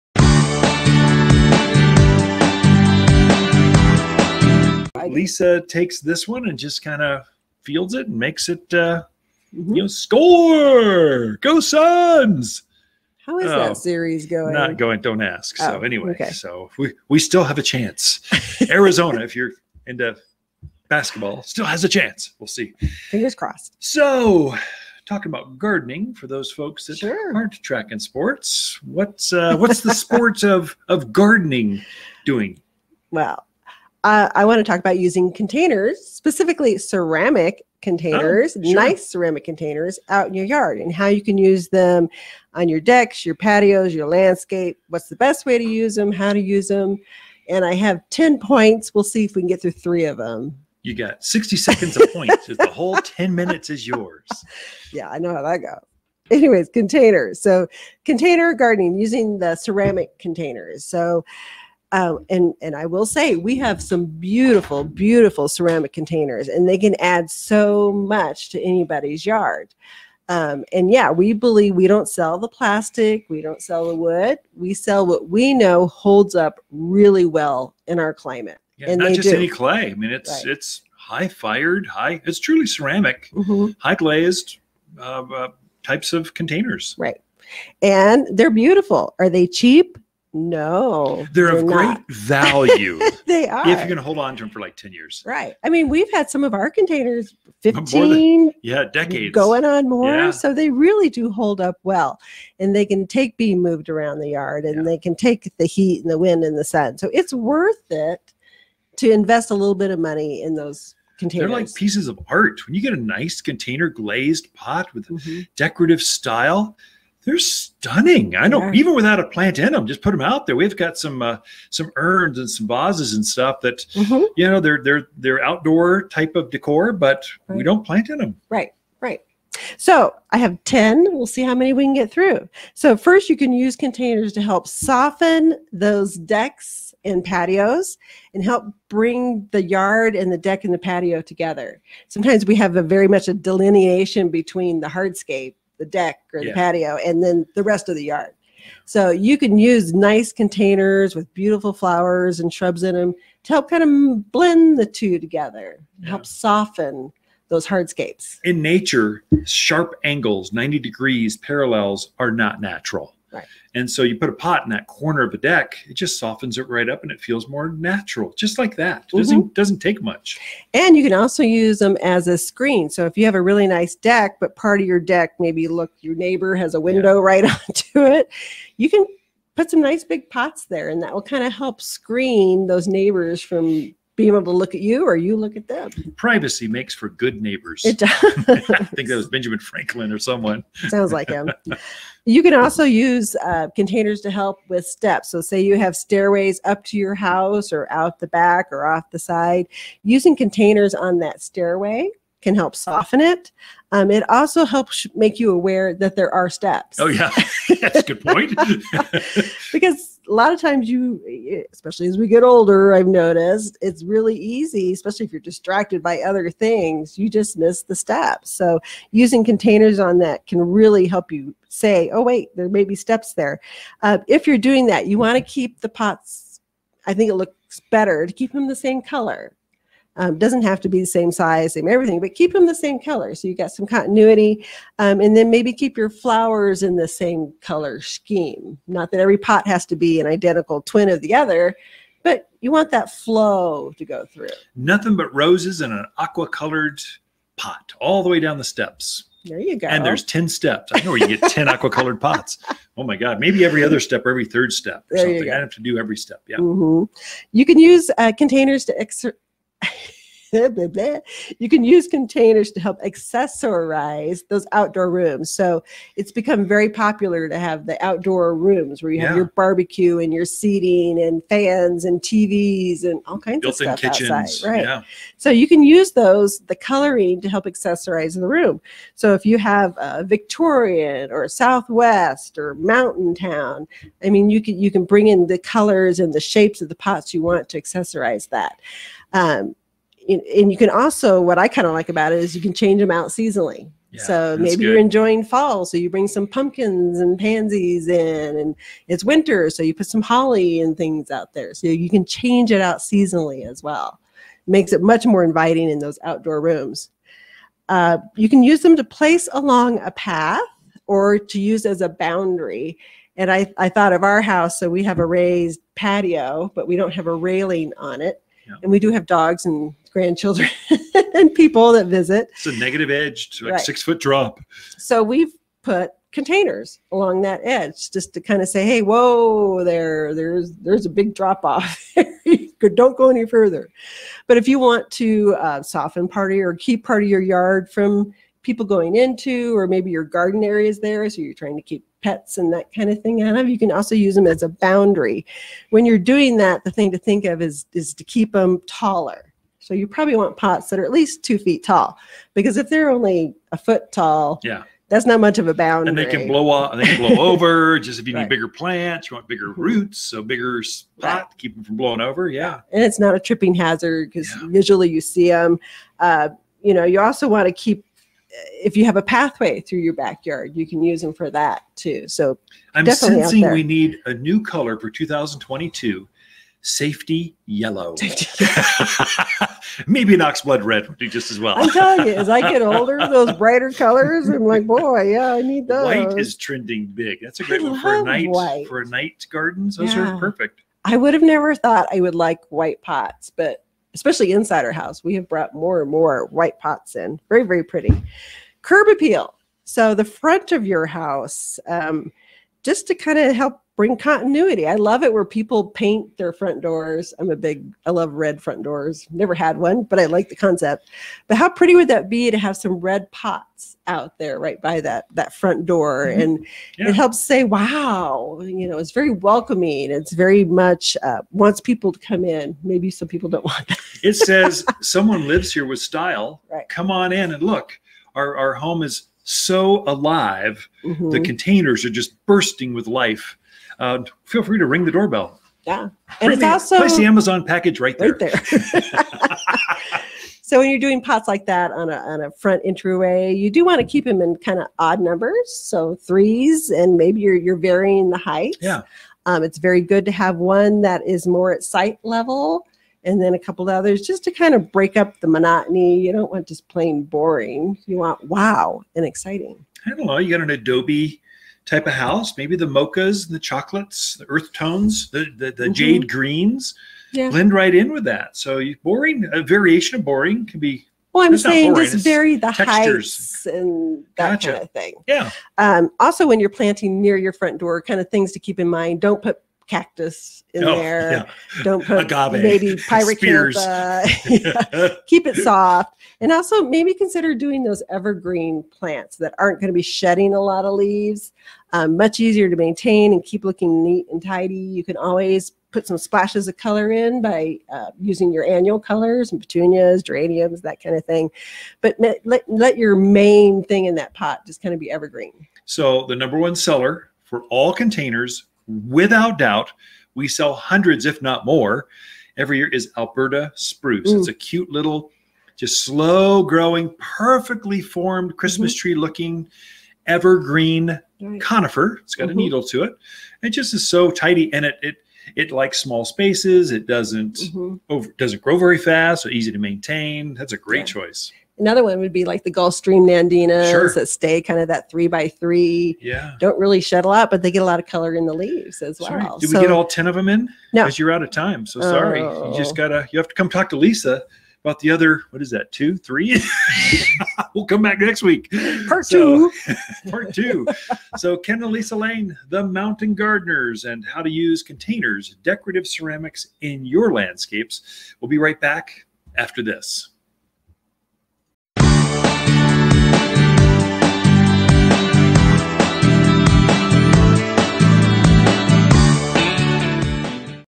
Behind the Scenes with the Mountain Gardeners - Lisa Designer Containers
The Best Ways to Use Containers
 - Soften and Bring a Deck or Patio into the Yard
    - Use Containers as a Privacy Screen
 - Use Containers on a Stairway
 - Help Accessorize Outside Rooms
     - Built-in Kitchens
     - Can Change Plants out Seasonally
 - Curb Appeal
     - Plant in Odd Numbers